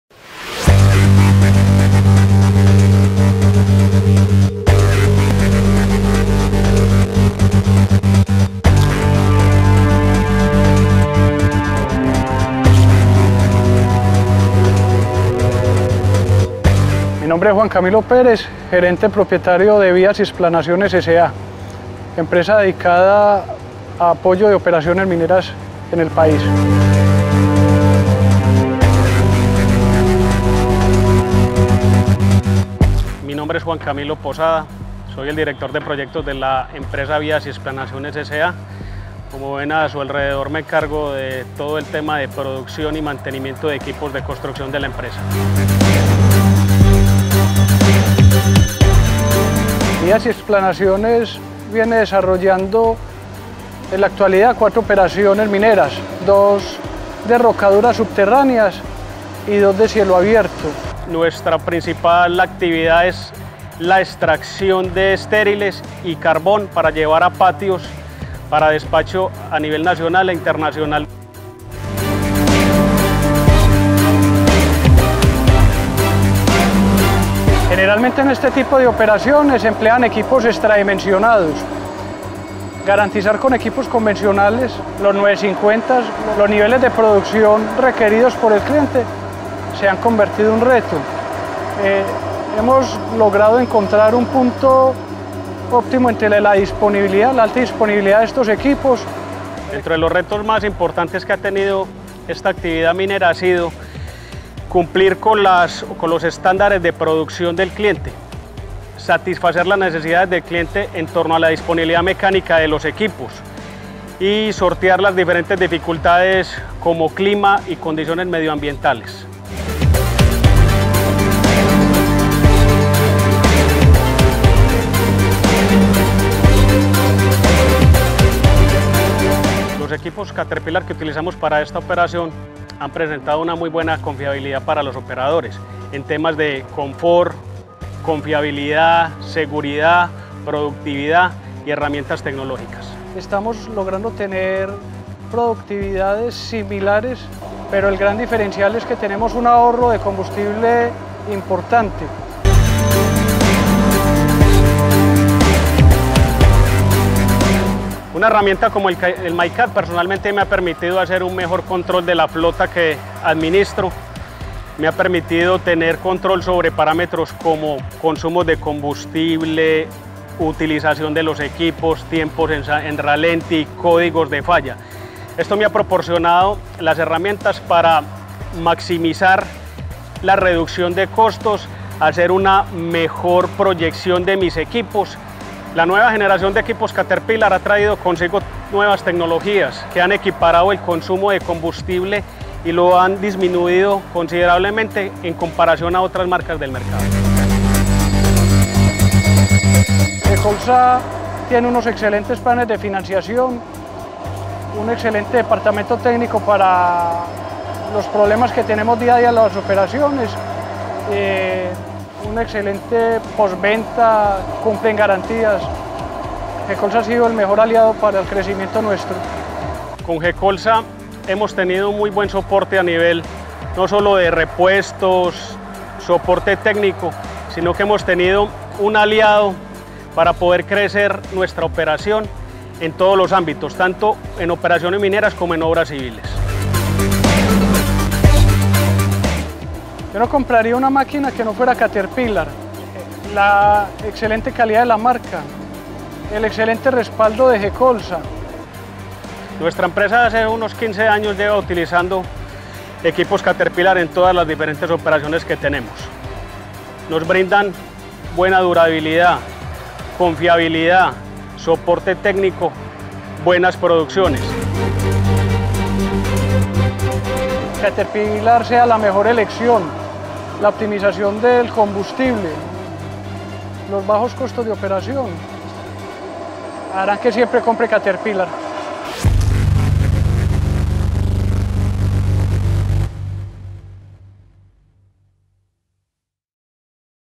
Caso de éxito Cargador 950GC
Conoce cómo nuestro cliente Vías y Explanaciones ha logrado altos estándares de producción gracias al Cargador 950GC, un equipo dotado de nuevas tecnologías que proporcionan mayor ahorro de combustible y otorgan confort y seguridad para los operadores.

Conoce más sobre este equipo a través de nuestra línea nacional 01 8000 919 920.